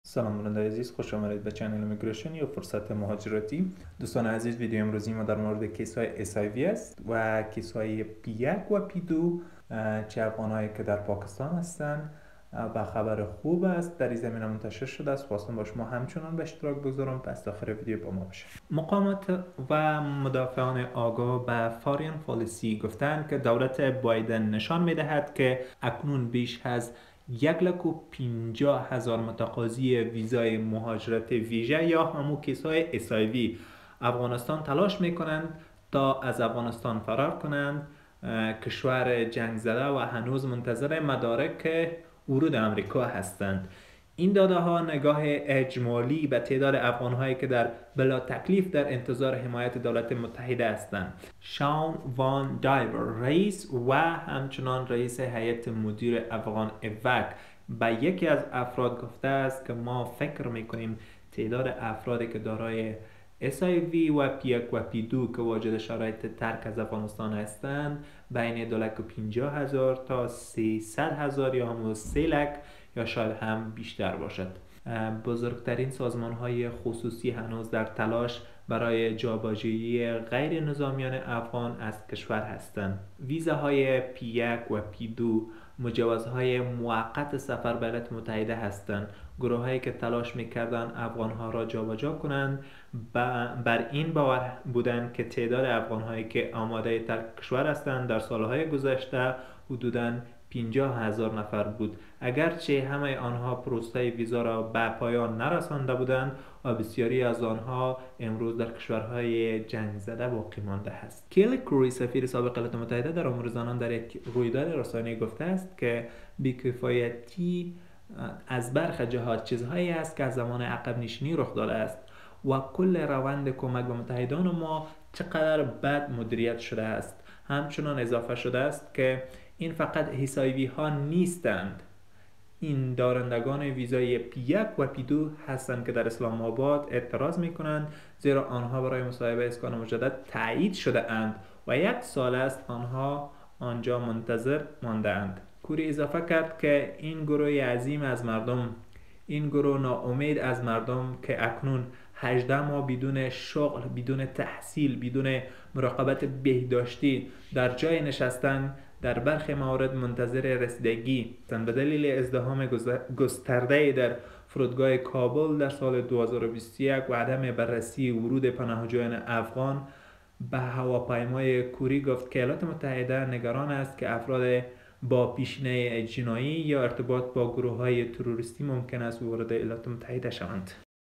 معلومات جدید درمورد کیس های SIV-P1-P2
Shawn VanDiver، رئیس و رئیس هیئت مدیره AfghanEvac، یکی از افراد، گفت: "ما فکر می کنیم تعداد افراد دارای SIV و P-1، P-2 که واجد شرایط ترک هستند بین 250000 تا 300000 یا شاید بیشتر باشد." بزرگترین سازمان های خصوصی هنوز در تلاش برای جابجایی غیرنظامیان افغان از کشور هستند. ویزاهای P-1 و P-2 مجوزهای موقت سفر به ایالات متحده هستند. گروه‌هایی که تلاش می‌کردند افغان‌ها را جابجا کنند، بر این باور بودند که تعداد افغان‌هایی که آماده ترک کشور هستند در سال گذشته حدود 50000 نفر بود، اگرچه همه آنها پروسه ویزا را به پایان نرسانده بودند و بسیاری از آنها امروز در کشور جنگ‌زده باقی مانده‌اند.